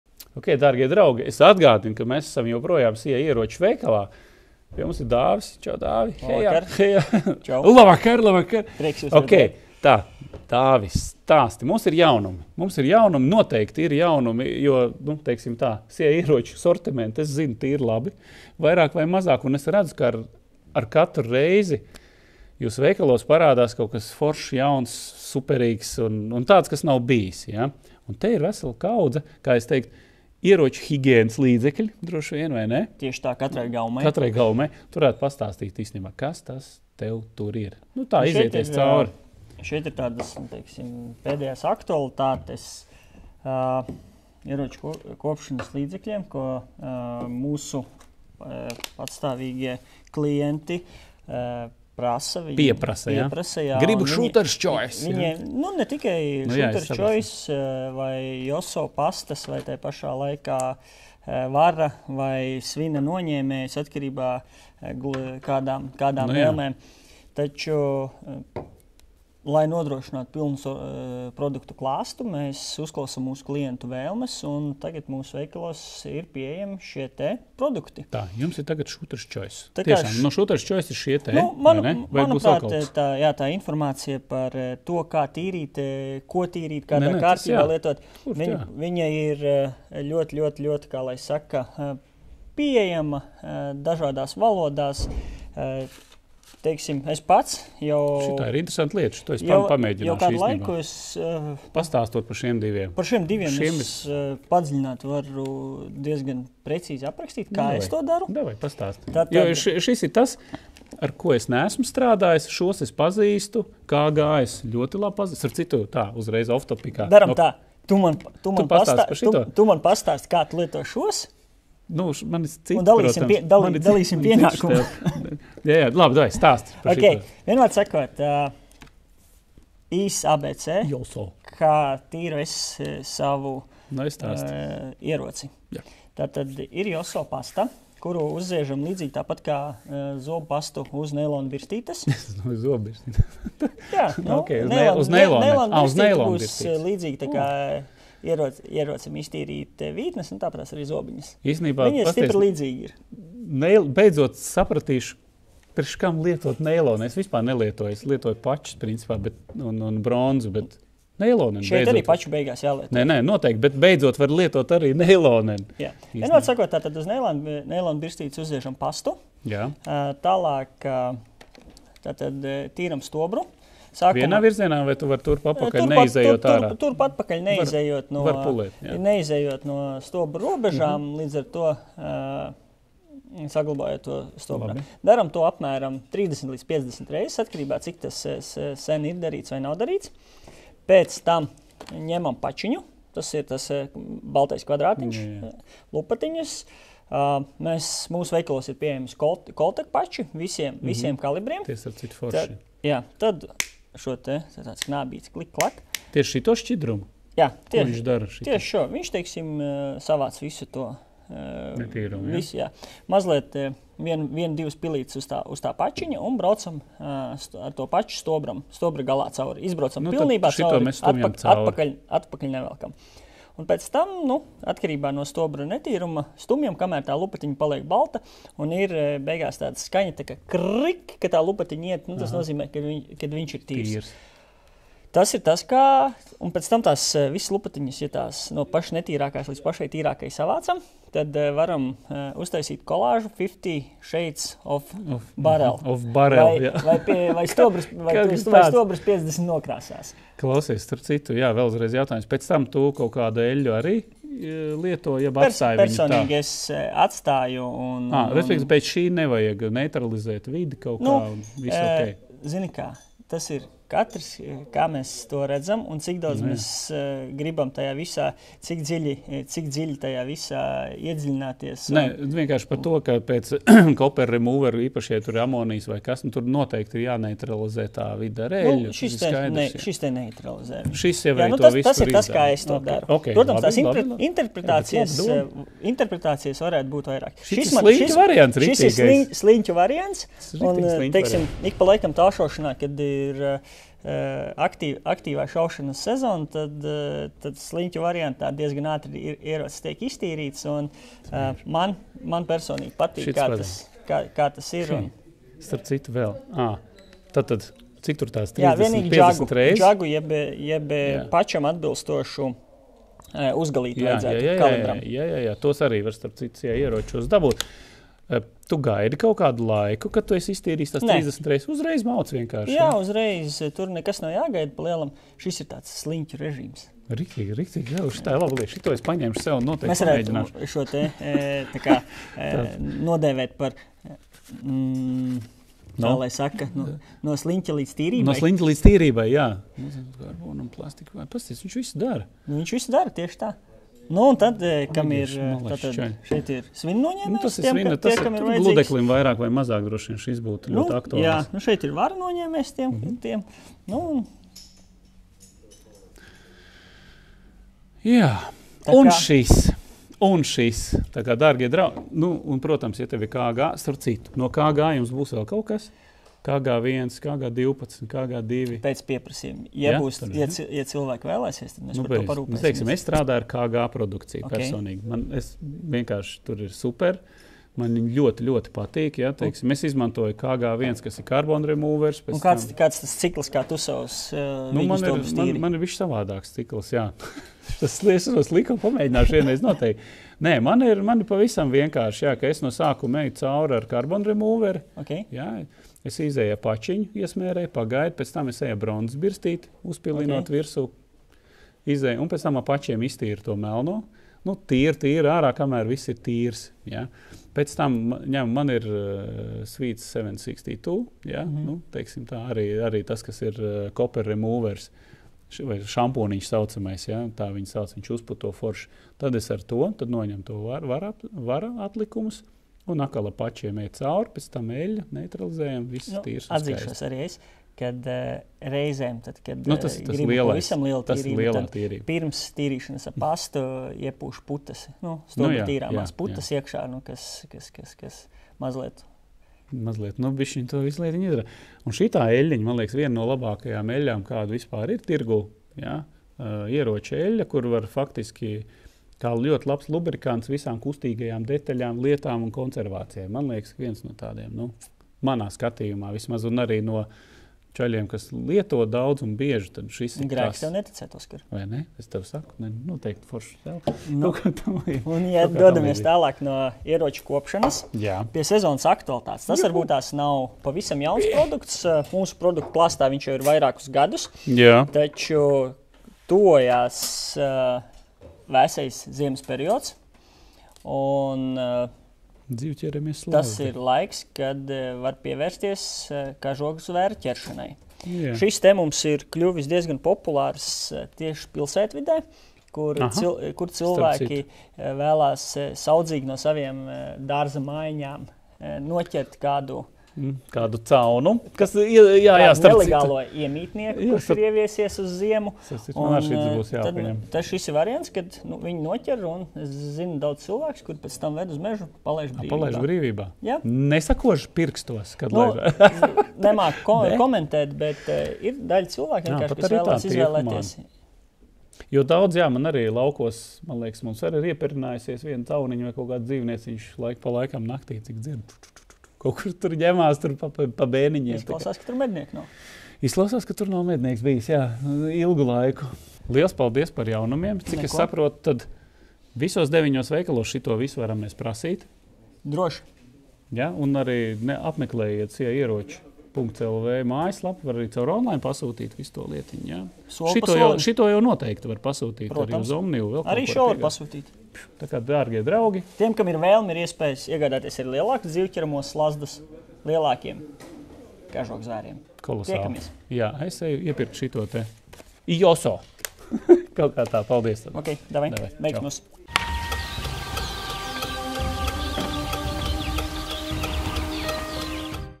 Ar ko tīrīt ieroci? Jaunumi no SIA "Ieroči"
SIA Ieroči pārstāvis Dāvis Zaube stāsta Oskaram Treiliham par to, kā un ar ko tīrīt ieroci. Veikalā jauni un interesanti produkti,. Tāpat noderīgas lamatas, piemēram, caunu medībām!

Pievienojieties šim kanālam, lai iegūtu piekļuvi privilēģijām.